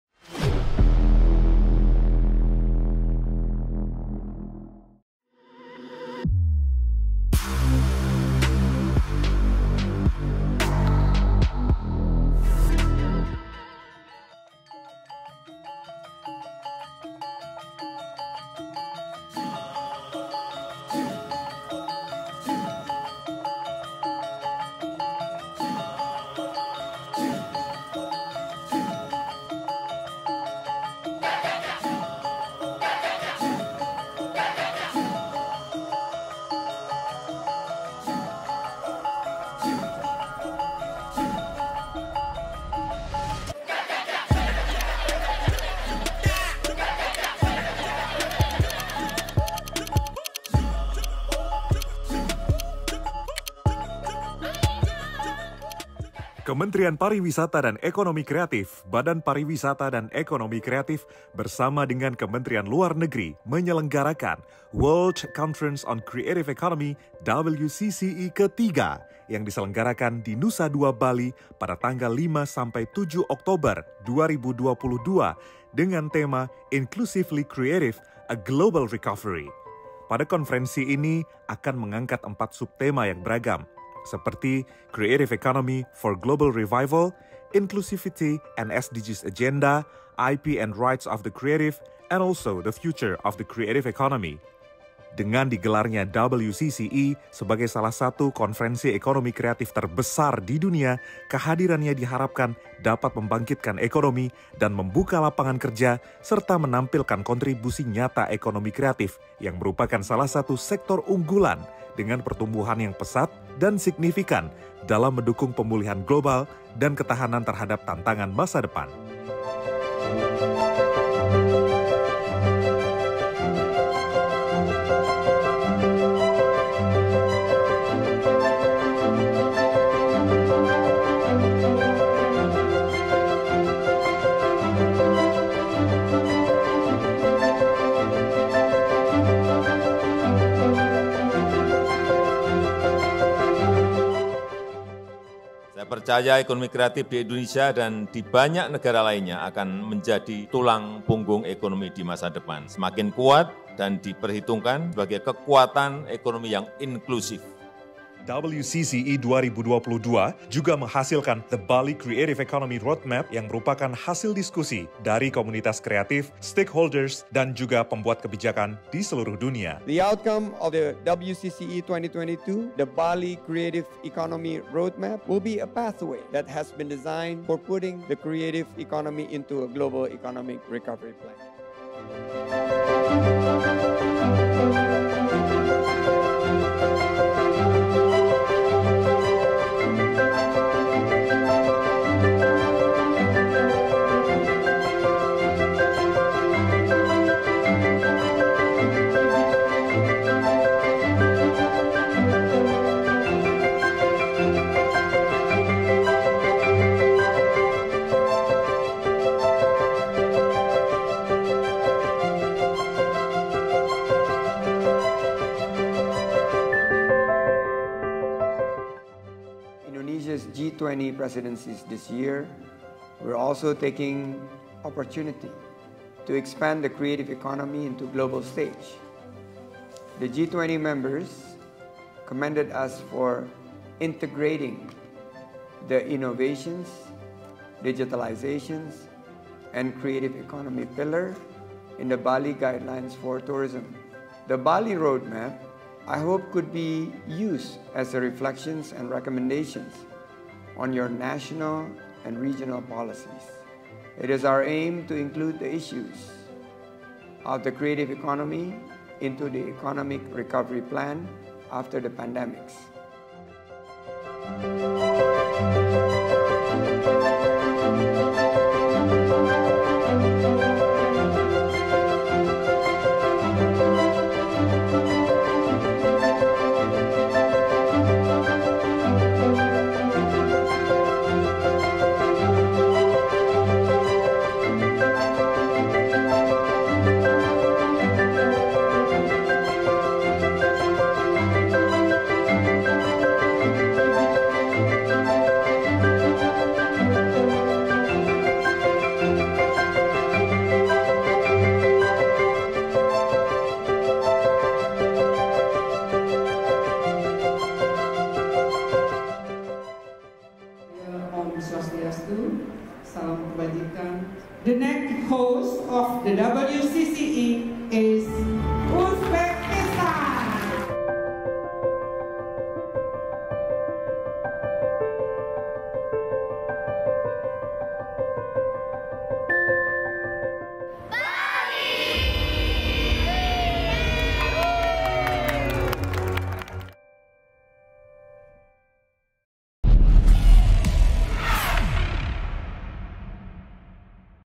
Ấn tượng lộ trình kinh tế sáng tạo Bali
Indonesia mới đây tổ chức thành công Hội nghị Thế giới 2022 về Kinh tế Sáng tạo (WCCE) tại Bali. Hội nghị bao gồm các chương trình nghị sự về nền kinh tế sáng tạo, Hội chợ triển lãm, Hội nghị Bộ trưởng và các chương trình hấp dẫn khác. Cũng tại đây, Indonesia đã đưa ra được Lộ trình Kinh tế Sáng tạo Bali - một chiến lược nhằm thúc đẩy nền kinh tế sáng tạo trong bối cảnh phục hồi kinh tế toàn cầu./.

(Nguồn: Dự án Sáng kiến trao đổi thông tin ASEAN 2022)